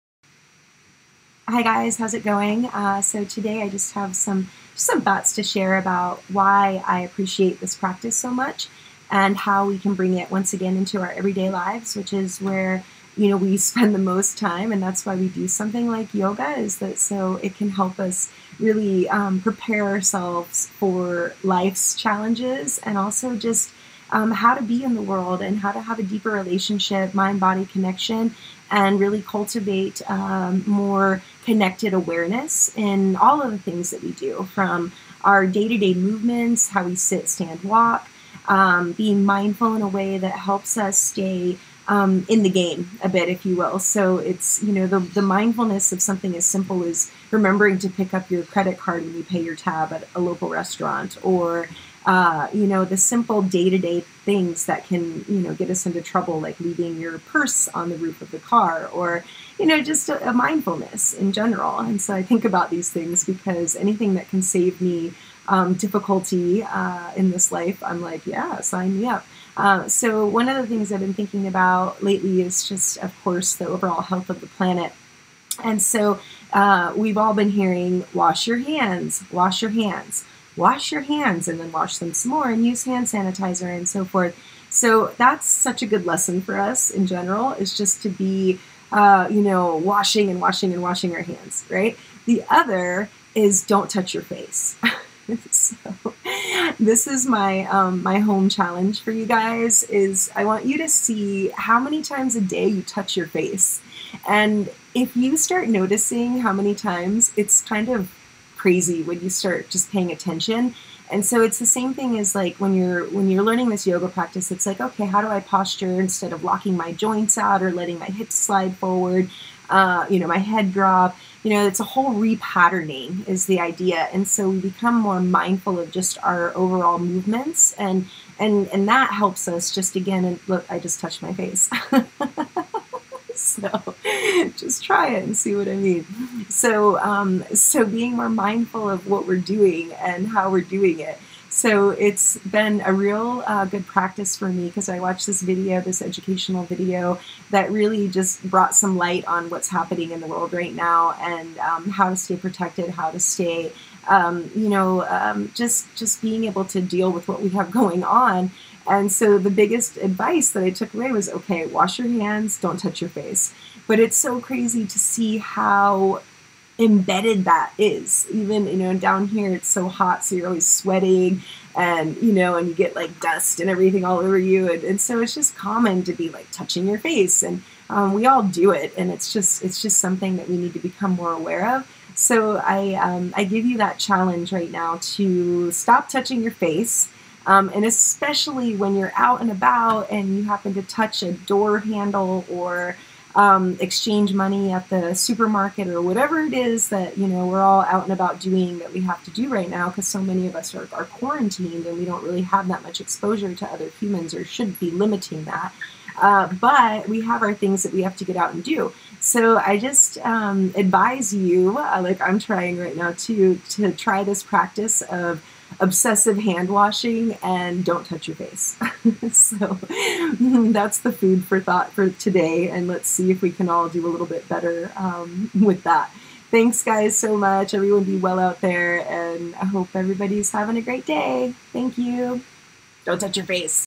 Wash your hands and don't touch your face!
This little video talks about being aware of our habits and how to shift old patterns. When we pay attention, we can save ourselves from a lot of BS.